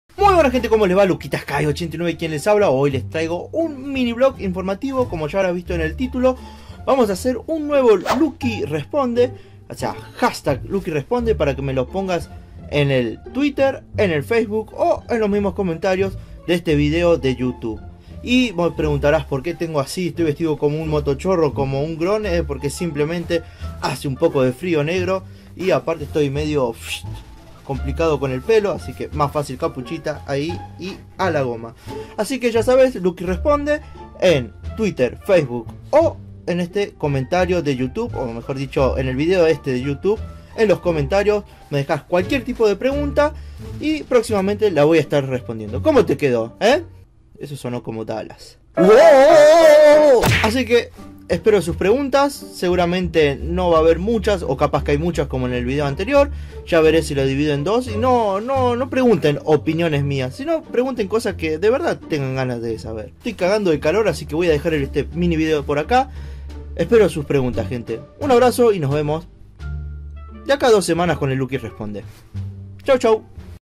Se viene otro LukiResponde?? | Preguntas y Respuestas ;)
Buenisimas gente, si quieren que haga otro LukiResponde usen este hastag en Facebook, Twitter o en los mismisimos comentarios de este video ;)
---SEGUIME !!!---